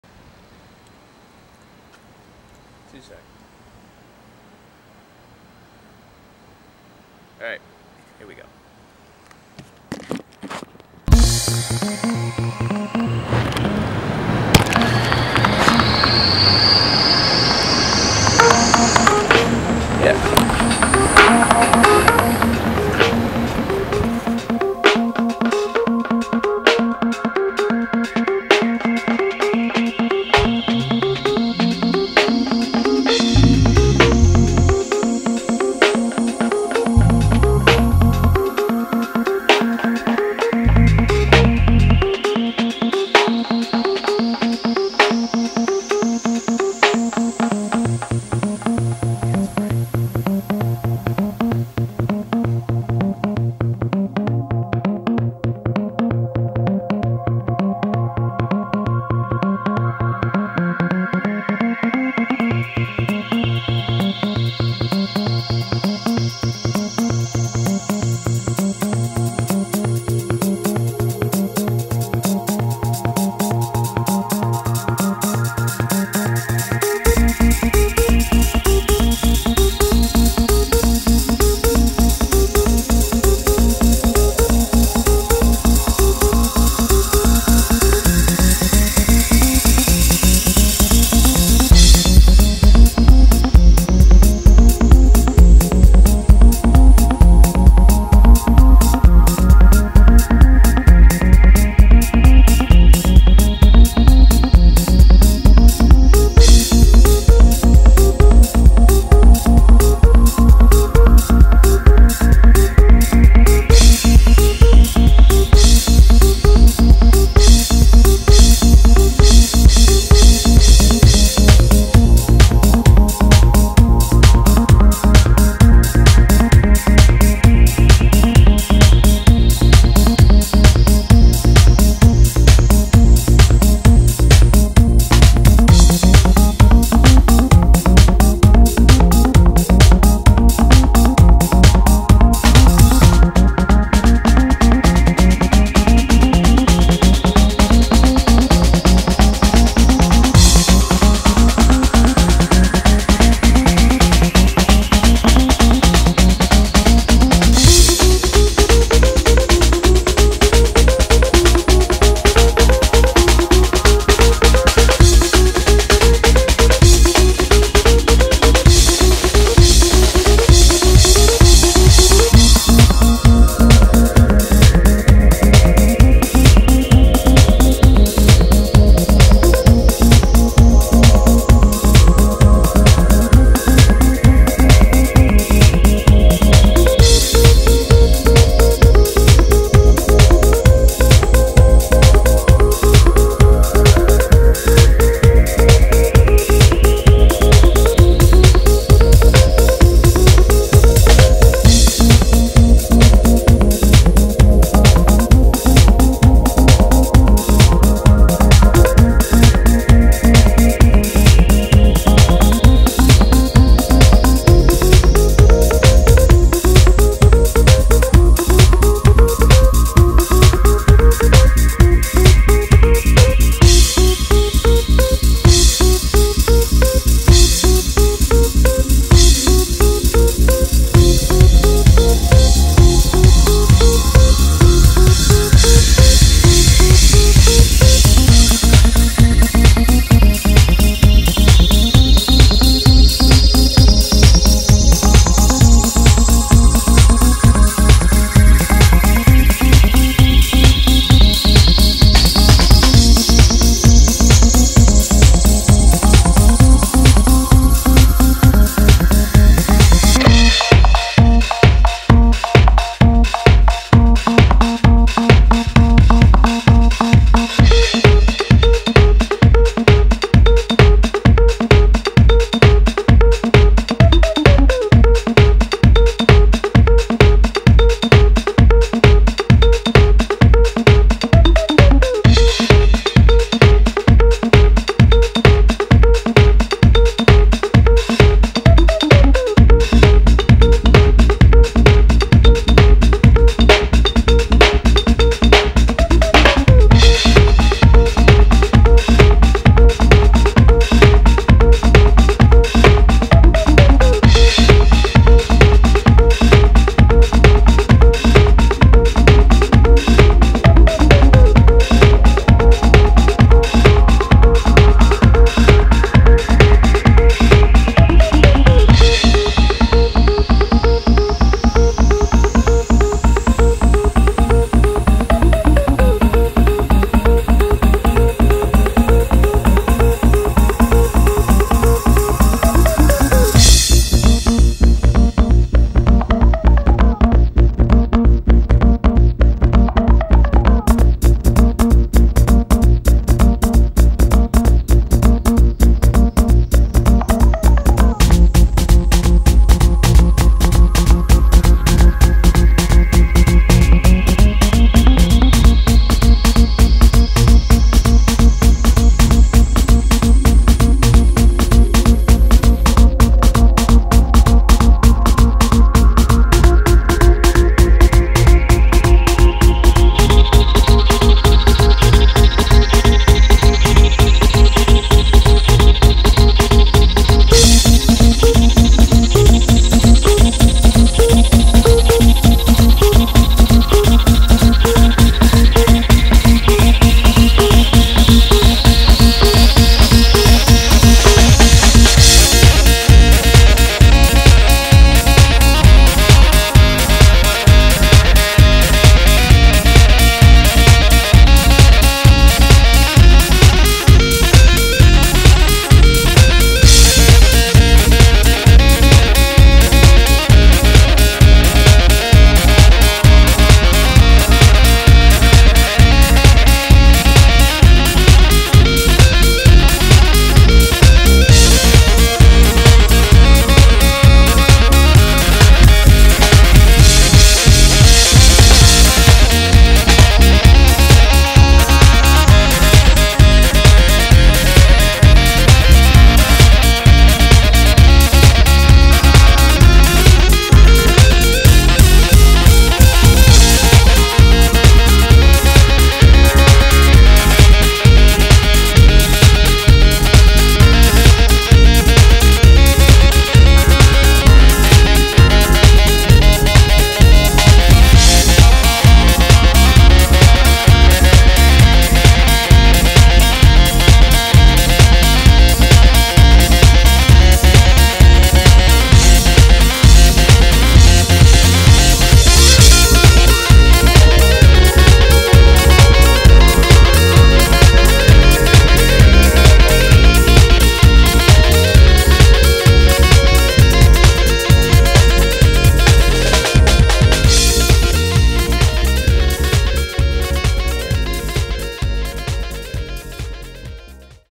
TRI-RIG Test 001
This is a test of a rig basically consisting of three SD cameras hard mounted to each other.